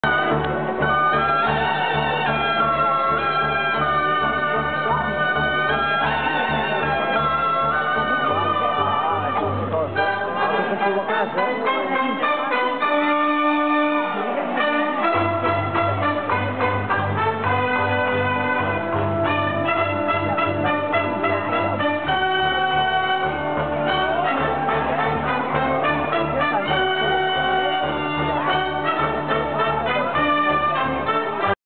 Monasterio de Montserrat sardana
Monasterio de Montserrat baile de sardana tocada por cobla (banda) 20 de junio de 2009

Cámara. Panasonic Lumix DMC-FX30